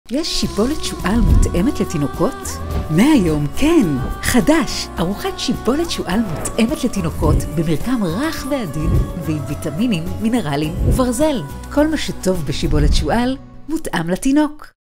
חדש ממטרנה! ארוחת שיבולת שועל מותאמת לתינוקות
לקחנו את כל הטוב שבשיבולת שועל ועשינו אותו מותאם לתינוק

 חדש ממטרנה! ארוחת שיבולת השועל של מטרנה  בטחינה דקה מבטיחה מרקם עדין המותאם לתינוקך, ודגנים שנבחרו בקפידה להכנת ארוחת דגנים טעימה  ומזינה, תוך עמידה בתקני האיכות הקפדניים של מזון תינוקות.

מטרנה ברשתות החברתיות:
מטרנה - פייסבוק